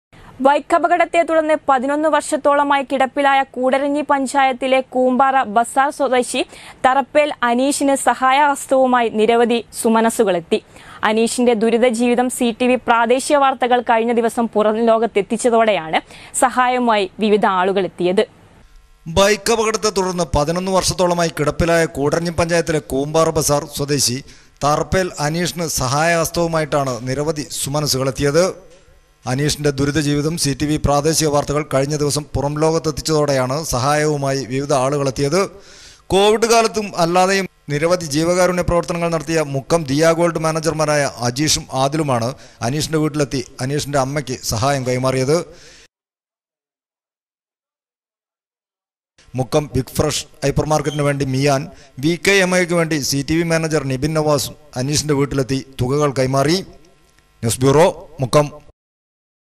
കൂമ്പാറ ബസാർ സ്വദേശി തറപ്പേൽ അനീഷിന് സഹായ ഹസ്തവുമായി നിരവധി സുമനസ്സുകളെത്തി
ബൈക്കപകടത്തെ തുടർന്ന് പതിനൊന്നു വർഷത്തോളമായി കിടപ്പിലായ കൂടരഞ്ഞി പഞ്ചായത്തിലെ കൂമ്പാറ ബസാർ സ്വദേശി തറപ്പേൽ അനീഷിന് സഹായ ഹസ്തവുമായി നിരവധി സുമനസ്സുകളെത്തി. അനീഷിന്റെ ദുരിത ജീവിതം സി.ടി.വി.പ്രാദേശിക വാർത്തകൾ കഴിഞ്ഞ ദിവസം പുറം ലോകത്തെത്തിച്ചതോടെയാണ് സഹായവുമായി വിവിധ ആളുകളെത്തിയത്.